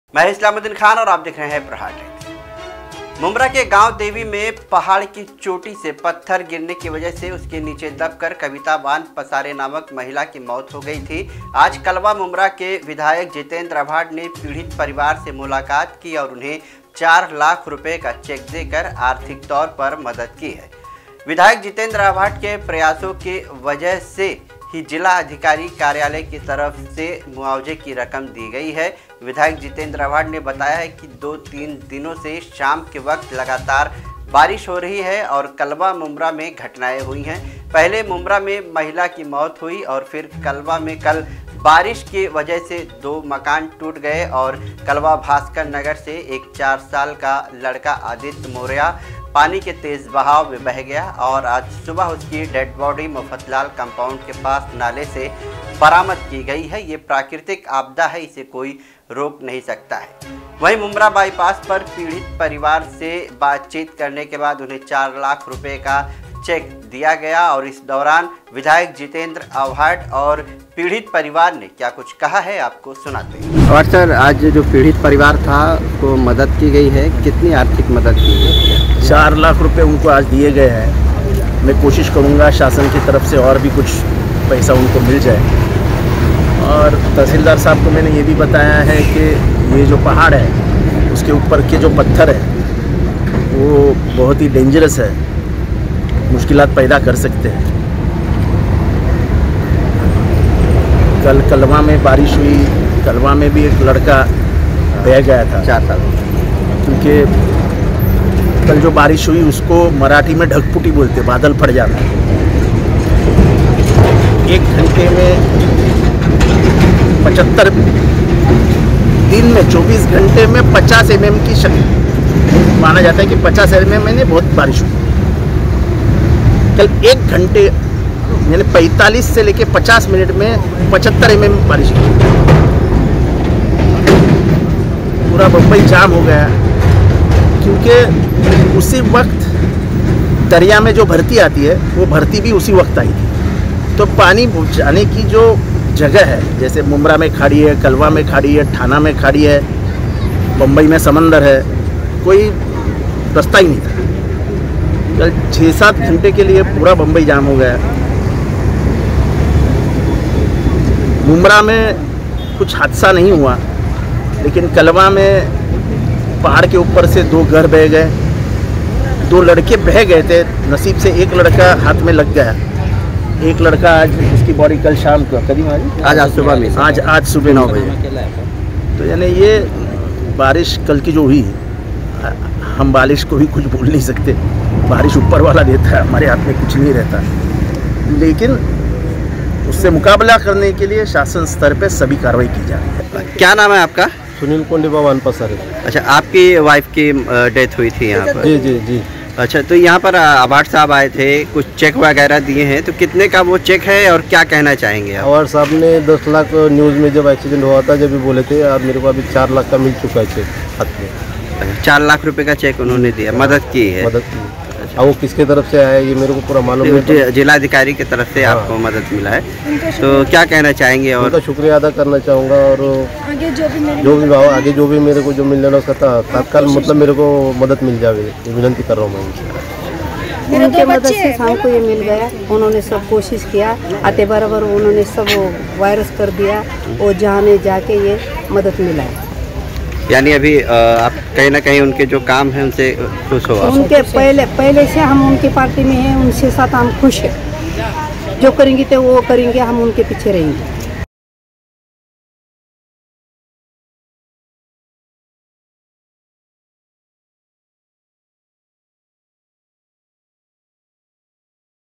𝐌𝐔𝐌𝐁𝐑𝐀 𝐍𝐄𝐖𝐒 : विधायक जितेंद्र आव्हाड ने पीढ़ीत परिवार को 4 लाख की मदद || Islamuddin Khan
मुम्ब्रा के गांवदेवी में पहाड़ की चोटी से पत्थर के नीचे दबकर कविता वान पसारे की मौत हुई थी आज कलवा मुम्ब्रा के विधायक जितेंद्र आव्हाड ने पीठित परिवार से मुलाकात को और उन्हें 4 लाख रुपये का चेक देकर आर्थिक तौर पर मदद की है जितेंद्र आव्हाड के प्रयासों की वजह से जिला अधिकारी के तरह से ये मुआवजा दिया गया है जितेंद्र आव्हाड ने बताया दो  तीन दिनों से शाम के वक्त बारिश से कलवा मुम्ब्रा में घटनाएं हुई पहले मुम्ब्रा में महिला की मौत हुई फिर कलवा में कल बारिश की वजह से दो घर टूट गए और कलवा भास्कर नगर से एक 4 साल का लड़का आदित्य मोरया बह गया था आज सुबह डेड बॉडी मोफ़त लाल कंपाउंड के पास नाले में मिली है बरामद की गई ये प्राकृतिक आपदा है इसे कोई रोक नही सकता ,मुम्ब्रा बाइपास  पर पीड़ित परिवार से बातचीत की और उन्हें चार लाख का चेक देने के बाद क्या कहा विधायक जितेंद्र आव्हाड,और पीड़ित परिवार ने